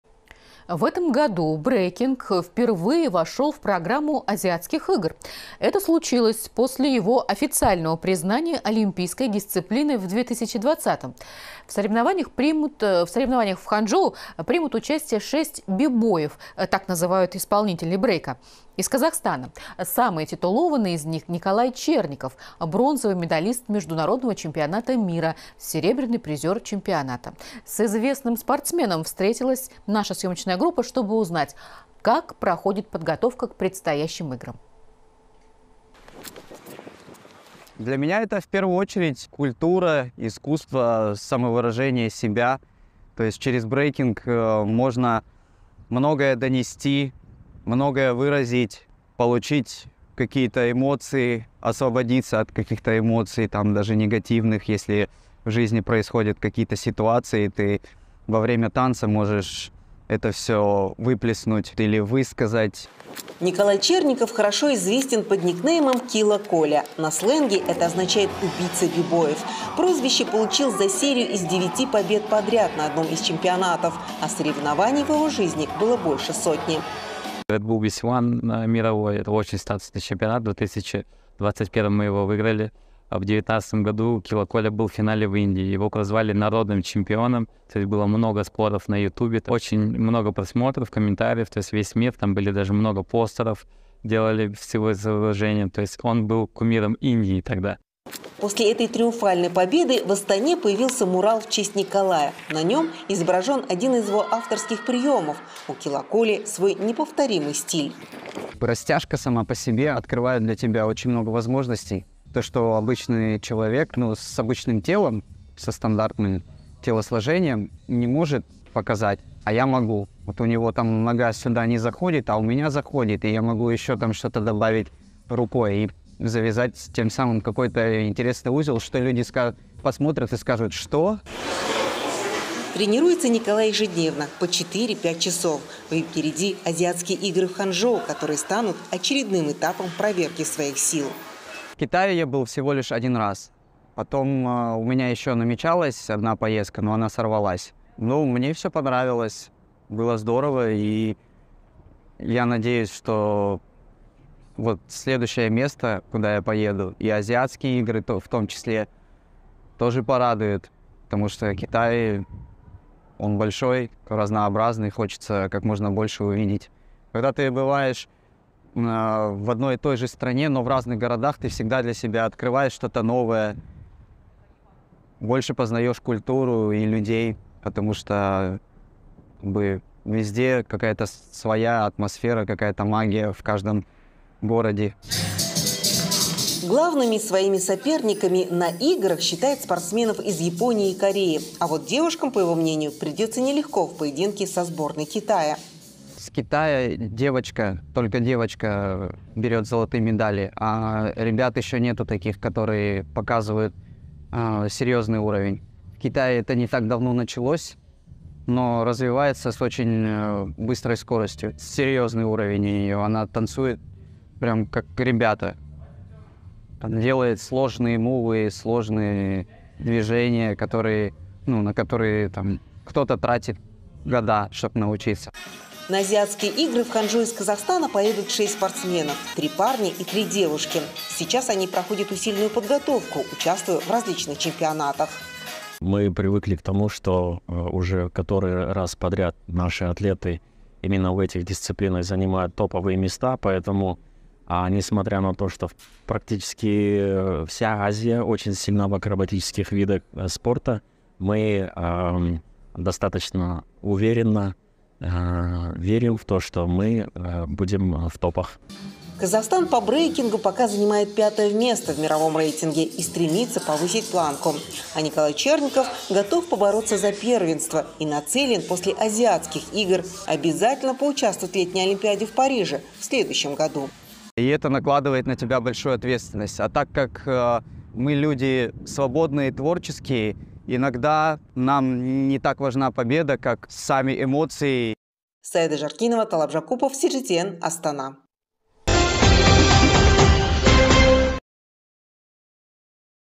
Казахстанские брейкдансеры готовятся к Азиатским играм-2023
В этом году брейкинг впервые вошел в программу Азиатских игр. Это случилось после его официального признания олимпийской дисциплиной в 2020-м. Что думают об этом виде спорта тутилованные исполнители брейка из Казахстана, - смотрите в нашем видео.